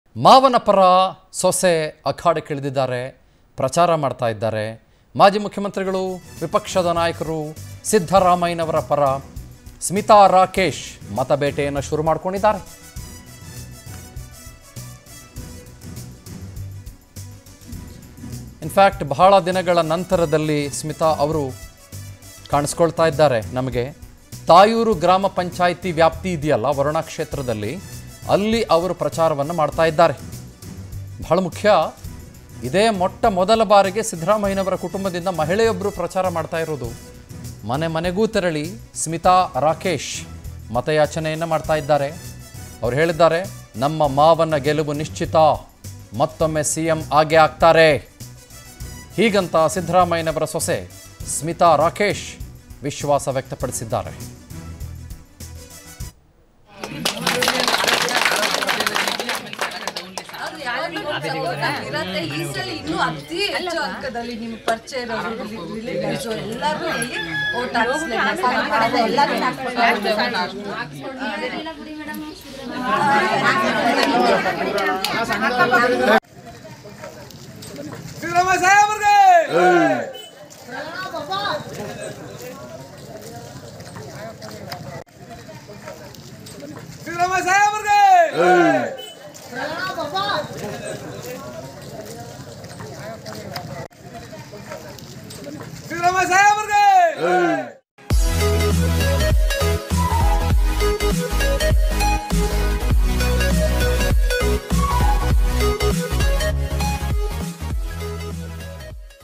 Siddaramaiah's Daughter-in-Law Smitha Rakesh : ಮಾವನ ಪರ ಪ್ರಚಾರದ ಅಖಾಡಕ್ಕಿಳಿದ ಸೊಸೆ | Varuna | Power TV
Siddaramaiah's Daughter-in-Law Smitha Rakesh : ಮಾವನ ಪರ ಪ್ರಚಾರದ ಅಖಾಡಕ್ಕಿಳಿದ ಸೊಸೆ | Varuna | Power TV News